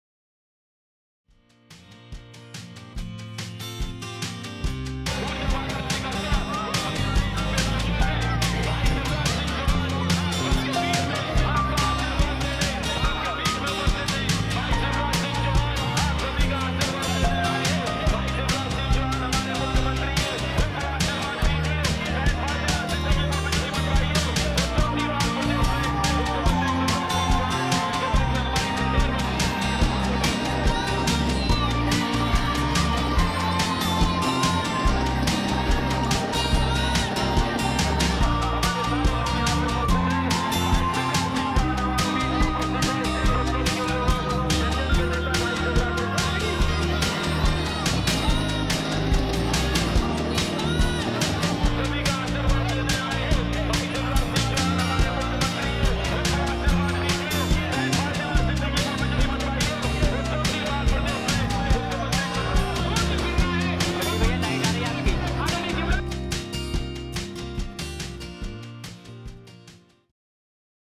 Watch: Shivraj Singh Chouhan conducts roadshow in Bhopal - Madhya Pradesh News
Bhopal (MP), Nov 23 (ANI): Madhya Pradesh Chief Minister Shivraj Singh Chouhan held massive roadshow in Indore. The roadshow was held ahead of the upcoming state elections. CM Chouhan later addressed the gathering.